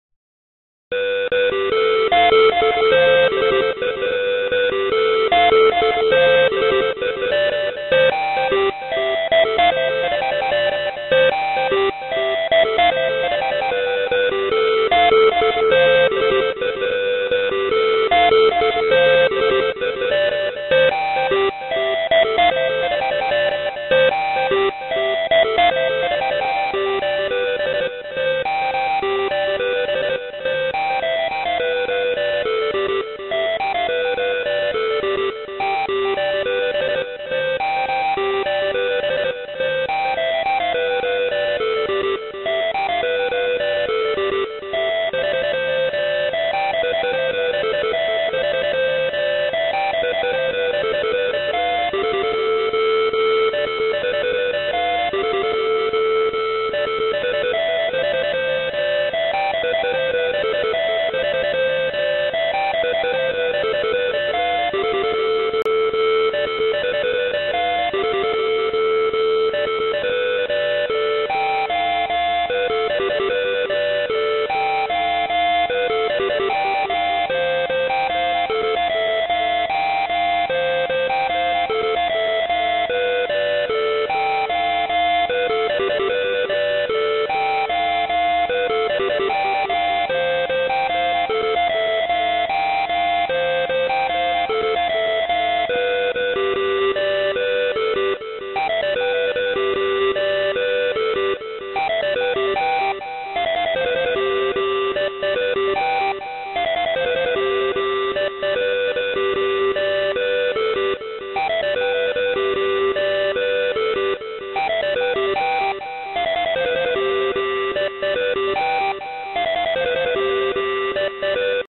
12 - Songs by God #6 (LoseThos) [2012]
Terry shows us songs co-created by him and Mr. God, part 6. AND, they now include cute animations!